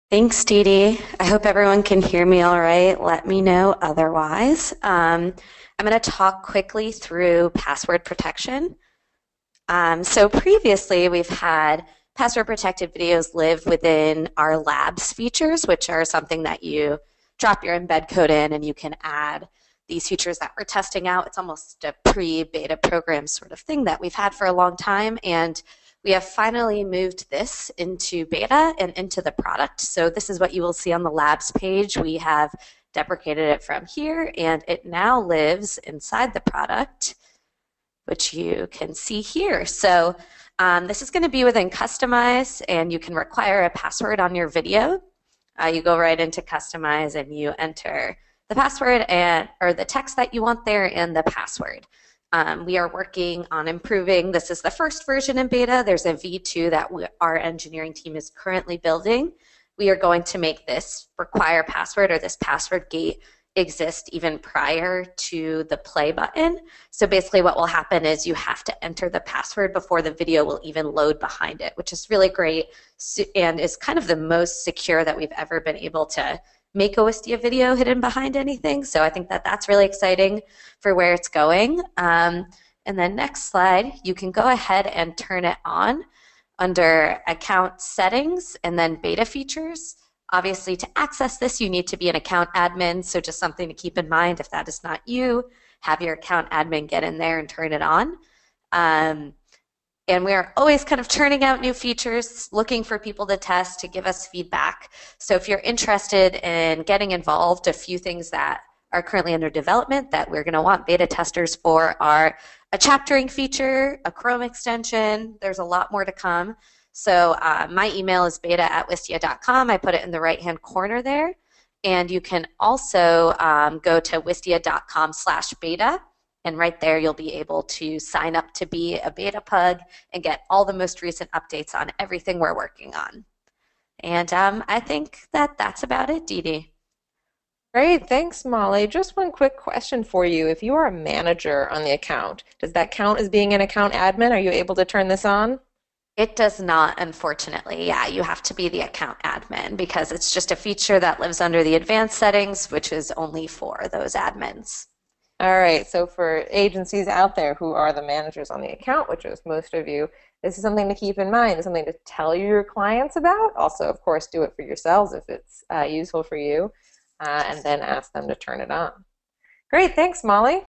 What's New In Wistia - Password Protected Videos
I recorded my screen using ScreenFlow 6 while attending the monthly Wistia Agency Partners webinar.